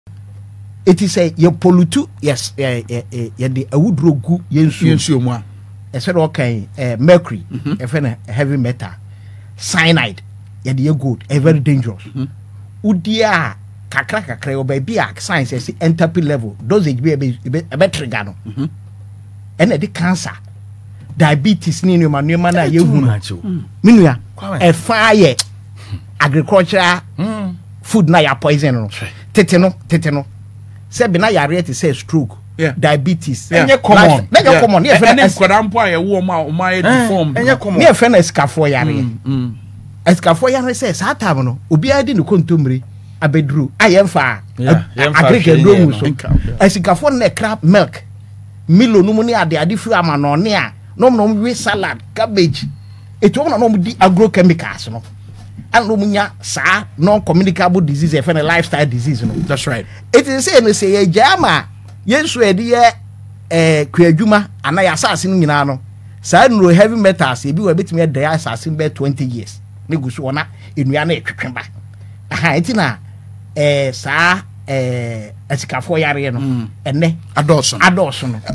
Illegal mining contributes to surge in cancer, diabetes, other health cases - NDC's Kwasi Etu-Bonde
Illegal mining contributes to surge in cancer, diabetes, other health cases - Former MP for Kintampo Kwasi Etu-Bonde